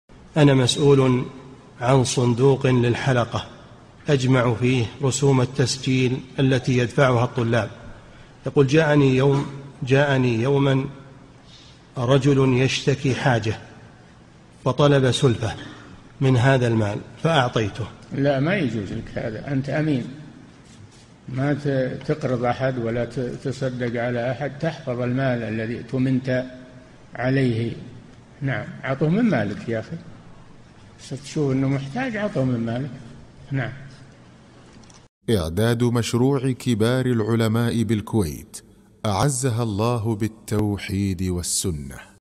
[2221 -3022] هل يجوز لمن يجمع رسوم التسجيل من الطلاب أن يقرض أحد المحتاجين من هذا المال؟
-"[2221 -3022] السؤال: وهذا سؤال قريبٌ منه يقول: أنا مسؤولٌ عن صندوقٍ للحلقة، أجمع فيه رسوم التسجيل التي يدفعها الطلاب، يقول: جاءني يوماً رجلٌ يشتكي حاجة وطلب سلفة من هذا المال فأعطيته.
الجَوَابُ:  لا ما يجوز لك هذا أنت أمين، ما تقرض أحد ولا تتصدق على أحد تحفظ المال الذي اؤتمنت عليه نعم عطه من مالك يا أخي بس تشوف أنه محتاج عطه من مالك.
للاشتراك في قناة الشيخ صالح الفوزان

للاستفسار والإقتراحات والدعم
رئيس الفريق
لمشروع كبار العلماء